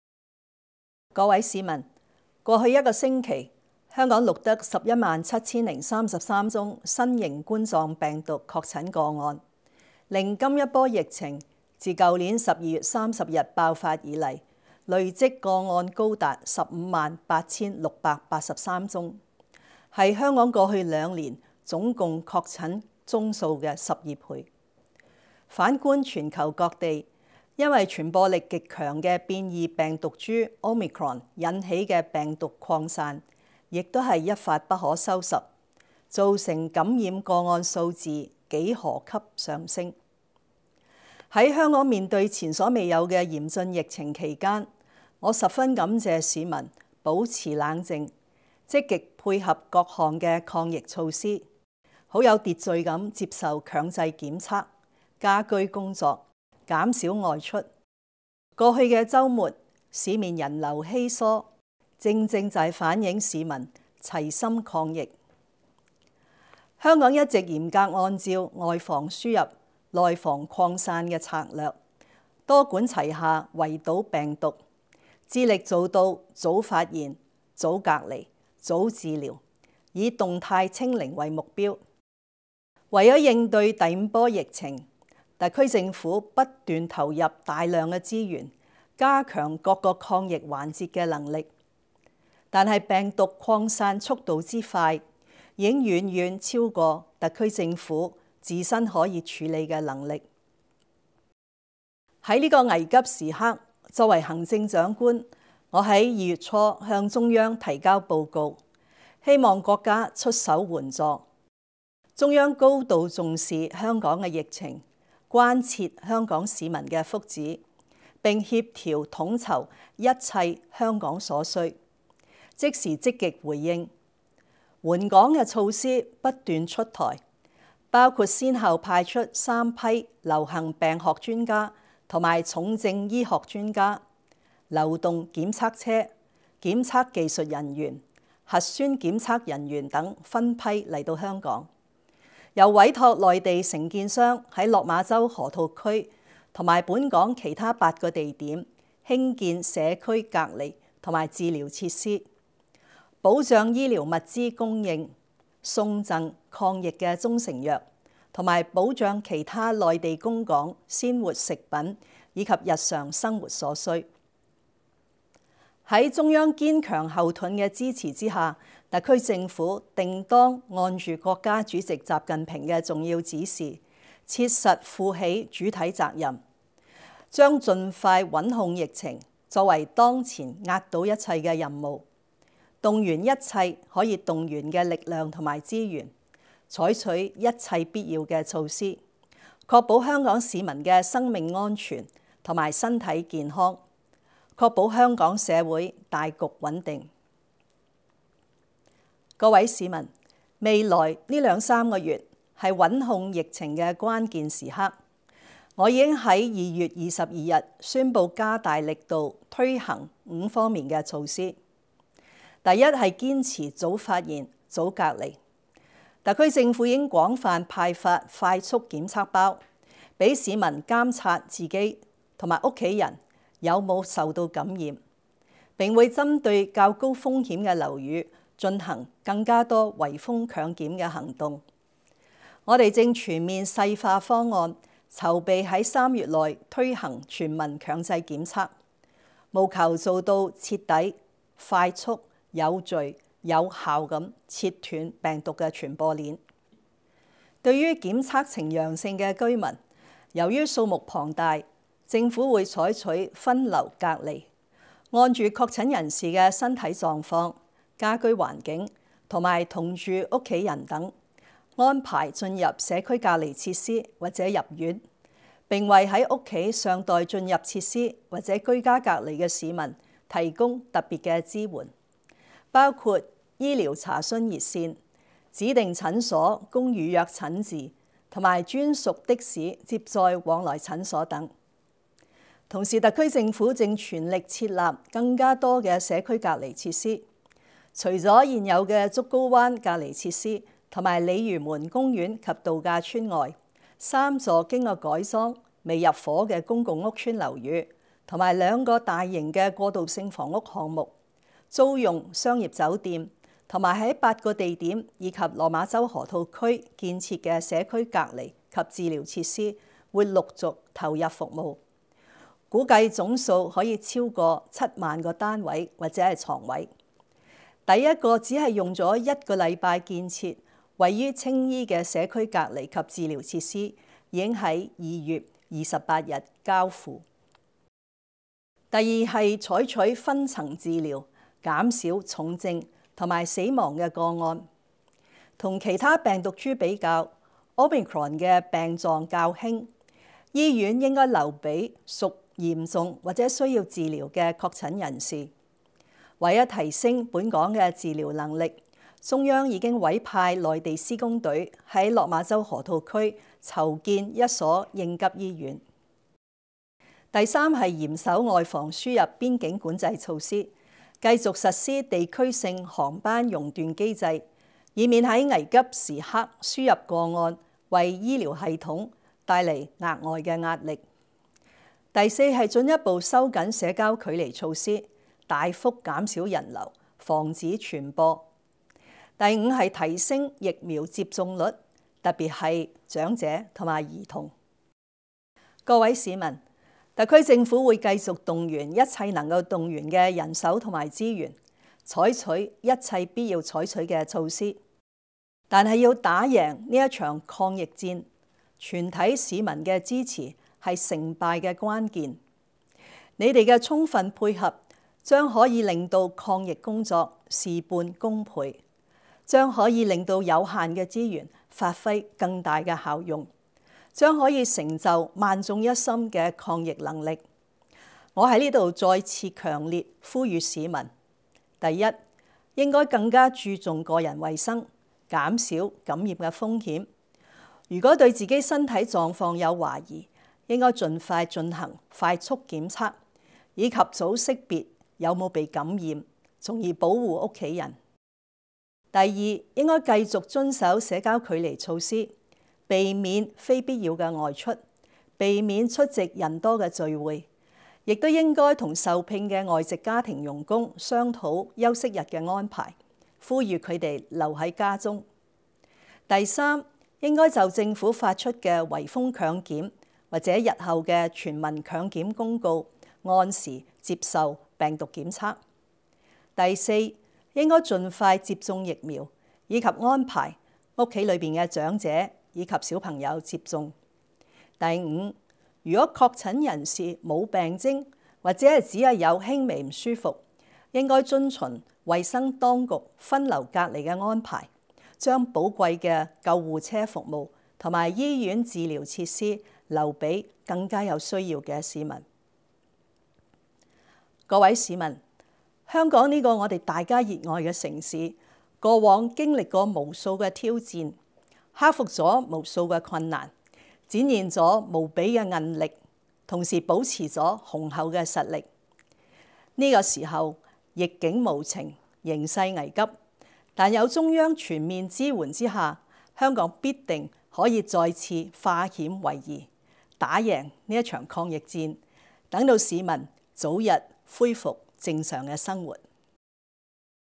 林鄭就抗疫發表視像講話：在中央全面支援下香港定能化險為夷
互相加油打氣！一定能戰勝！